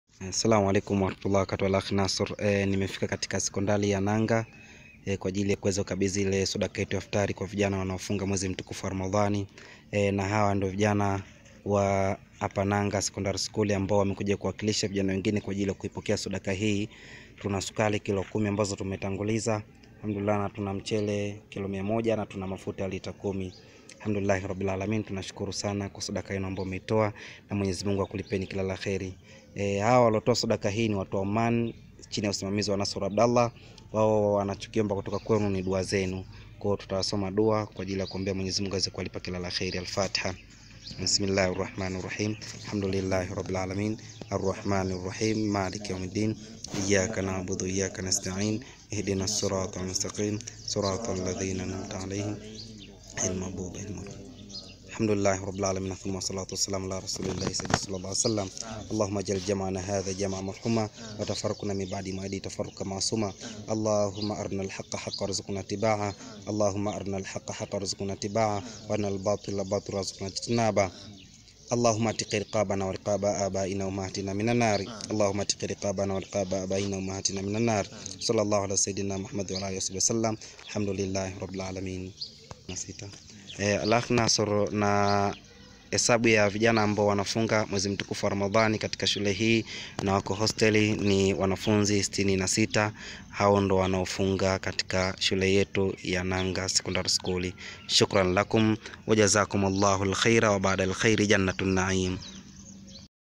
Ramadhan Charity 2021 Nanga Secondary School Igunga
Alkhair Community reaches to students in Nanga Secondary School for Ramadhan Charity